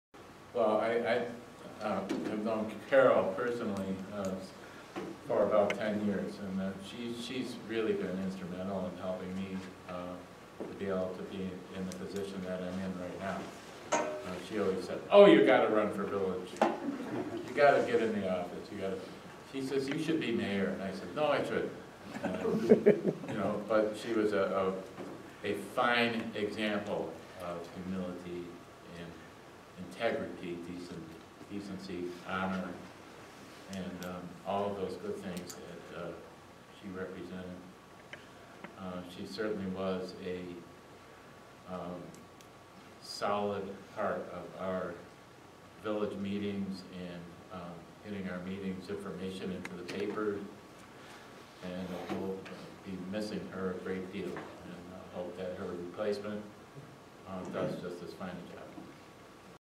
18 03 08 Recognition of Carol Klem Karl Laurer
Karl Lauer, memories of Carol Klem, March 8, 2018 Webster Village Board Meeting